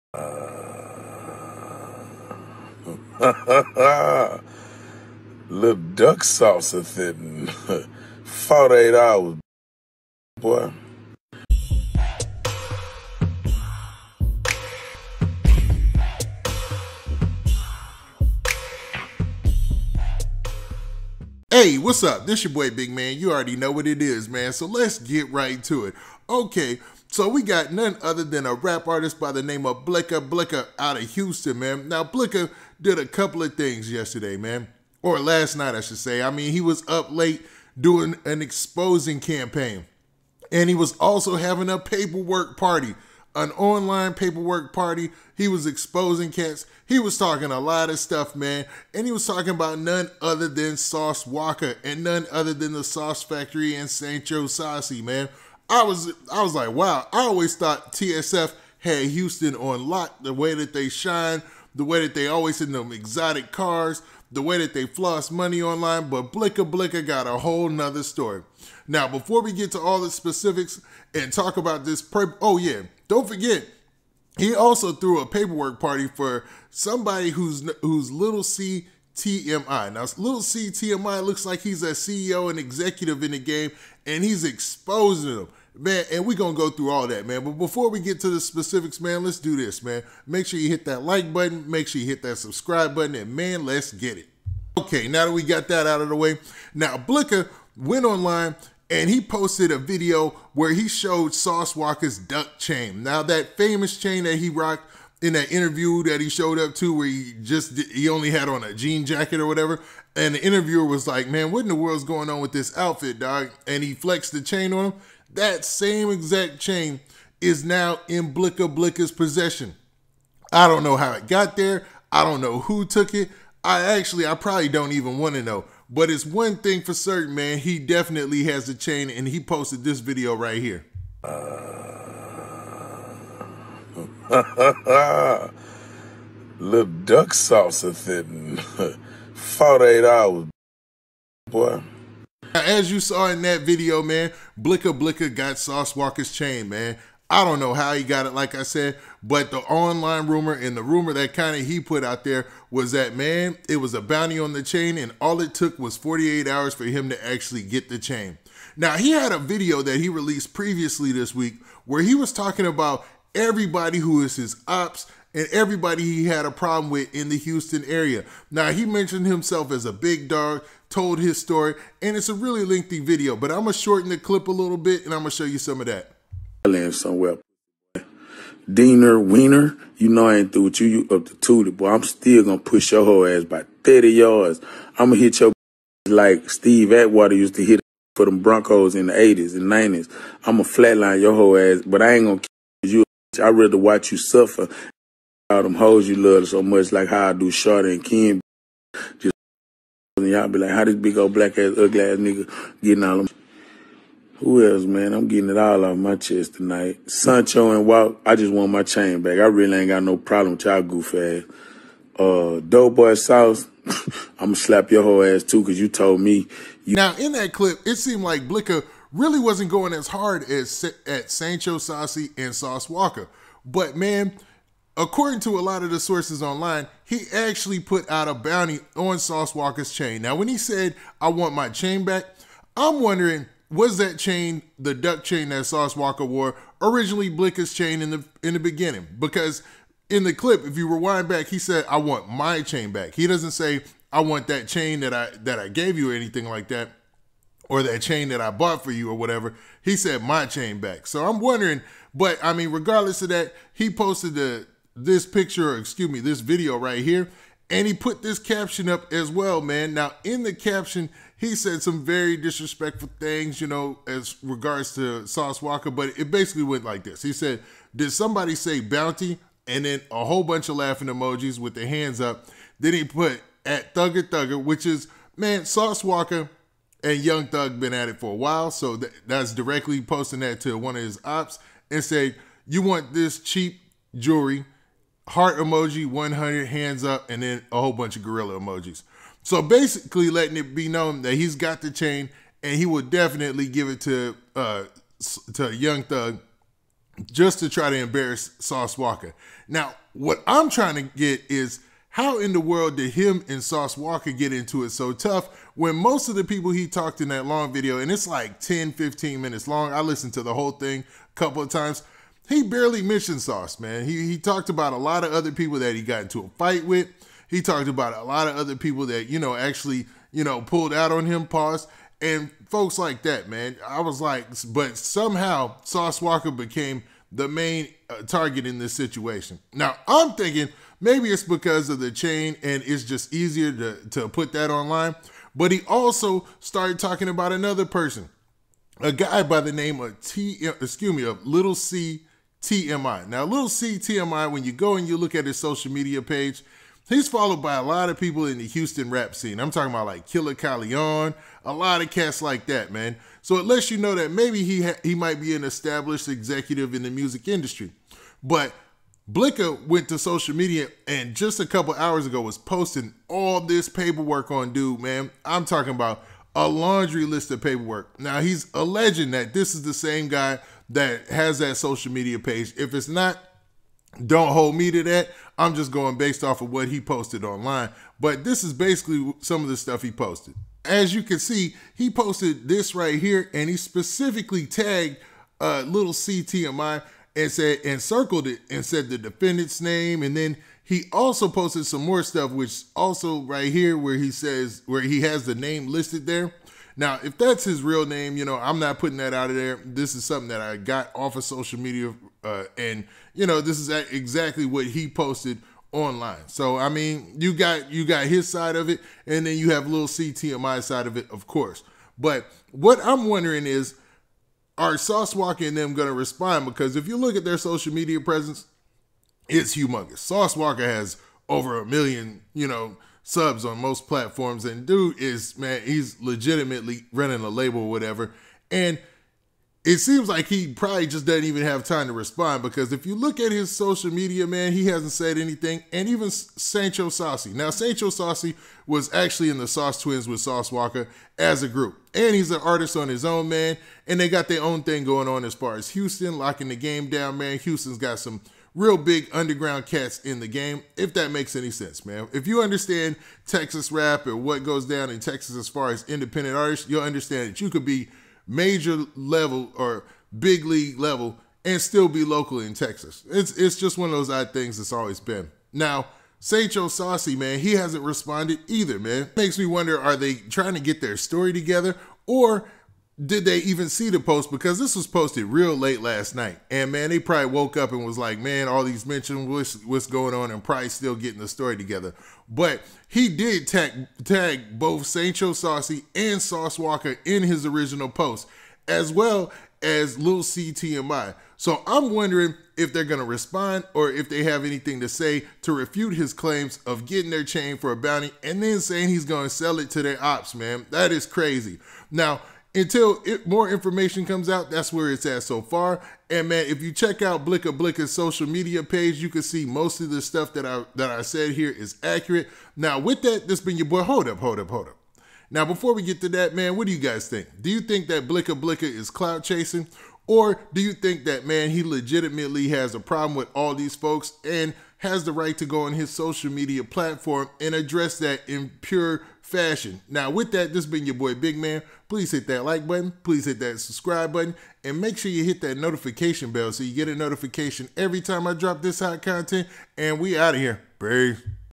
Rapper Blika Get's Sauce Walka's Chain 24 Hrs After Putting Out A Bounty!! Selling it to Young Thug?
Houston rapper Blika Blika took to social media to taunt Sauce Walka and TSF after putting out a bounty for Sauce Walka's chain then receiving the chain in less than 24 hours. He is threatening to sell it to Walka's industry Opps including Young Thug. He also tries to expose other Houston artist with paperwork.